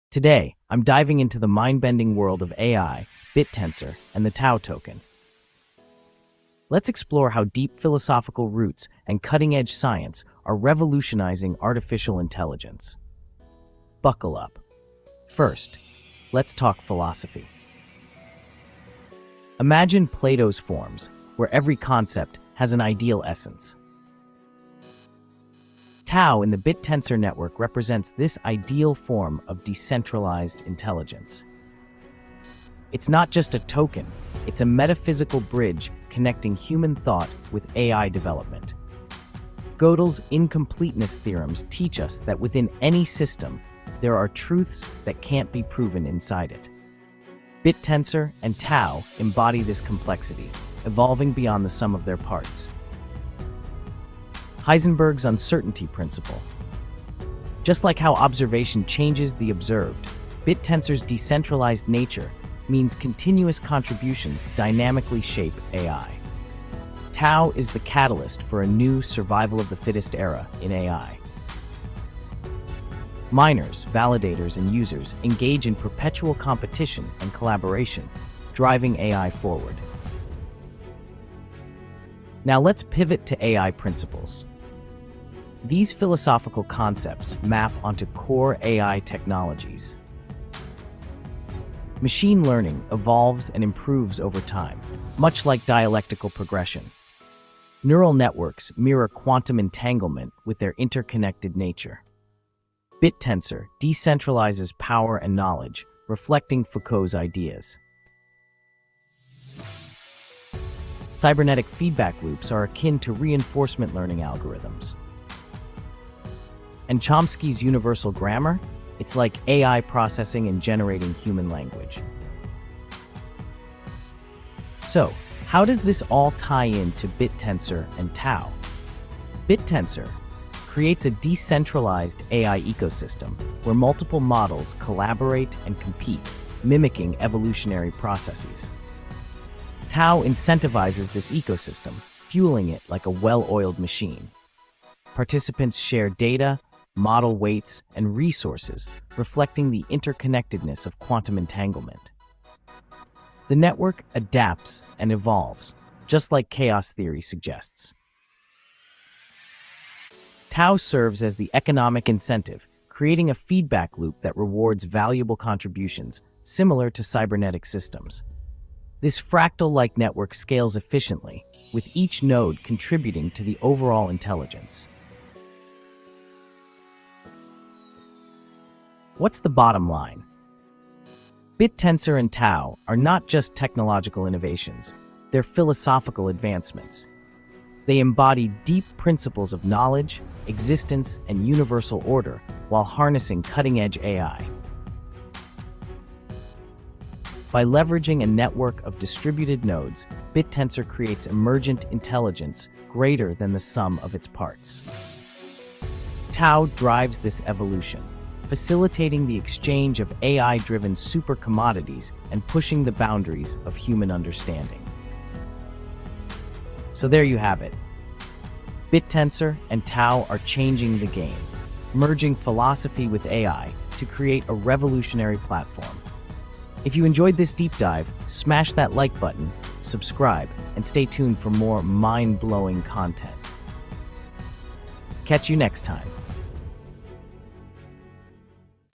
🤯 Plato, Quantum Physics, and $TAO? This video will blow your mind! 🚀
The Mind-Bending Truth About $TAO: How Ancient Philosophy Predicts the AI Revolution

Discover how age-old wisdom and cutting-edge science are colliding in the Bittensor network.
🧠 Unlock the secrets of decentralized AI
💡 Learn why $TAO is more than just a token
🌌 See the future of technology unfold before your eyes

👇 Watch now and prepare to have your reality altered! 👇

Dive deep into the philosophical and scientific foundations of Bittensor and $TAO in this mind-expanding 4-minute journey. Uncover how concepts from Plato to quantum mechanics are shaping the future of decentralized AI. This video explores the intricate connections between ancient wisdom, modern science, and cutting-edge technology, revealing how $TAO is at the forefront of a technological revolution.

Topics covered:
- Emergent intelligence and Gödel's incompleteness theorems
- Quantum entanglement and AI interconnectedness
- Darwinian evolution in machine learning
- Fractal nature of decentralized networks

Perfect for tech enthusiasts, philosophers, and anyone curious about the future of AI and cryptocurrency.